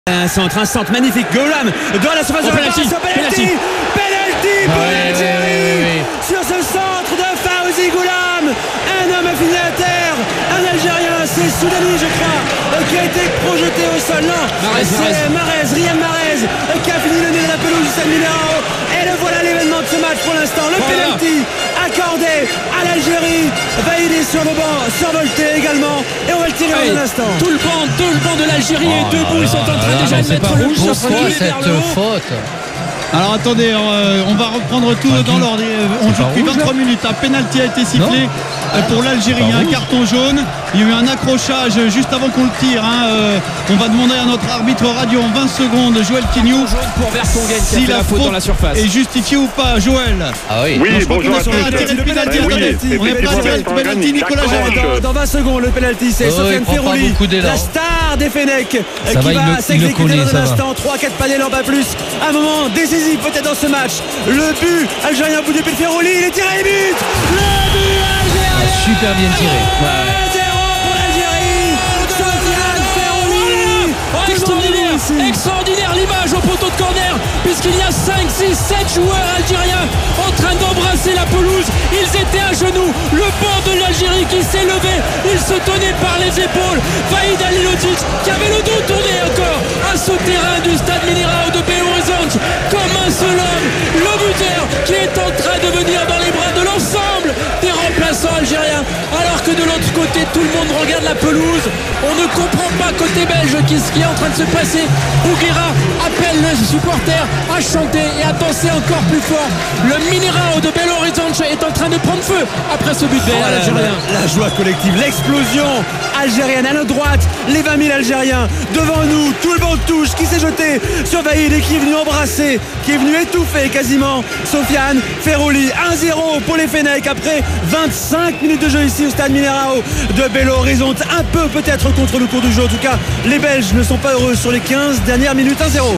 Belgique - Algérie : Le penalty de Feghouli (0-1) avec le son RMC Sport !
Revivez l'émotion du penalty de Sofiane Feghouli qui a ouvert le score face aux diables rouges à la 25ème minutes, contre le court du jeu, dans ce premier match du groupe H de la Coupe du Monde 2014, avec les commentaires RMC Sport.